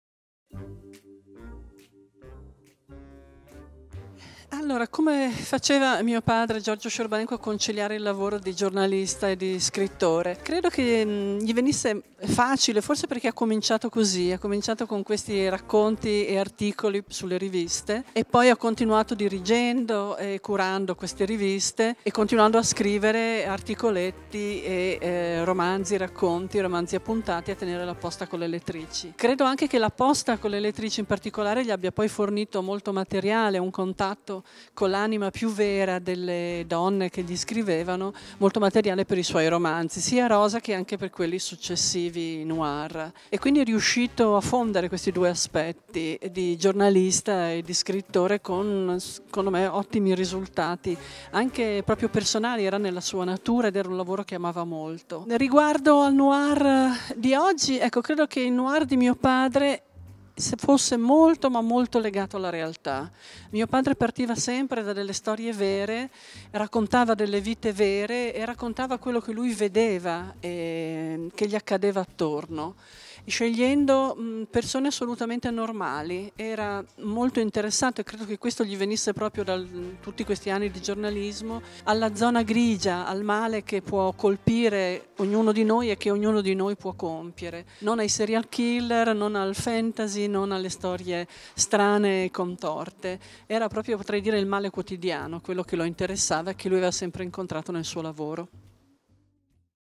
Noir in Festival 2019 - Interista a Cecilia Scerbanenco
In occasione del Noir in Festival 2019, abbiamo intervistato Cecilia Scerbanenco, figlia del celebre giallista, che ci ha raccontato i processi creativi alla base della scrittura del padre.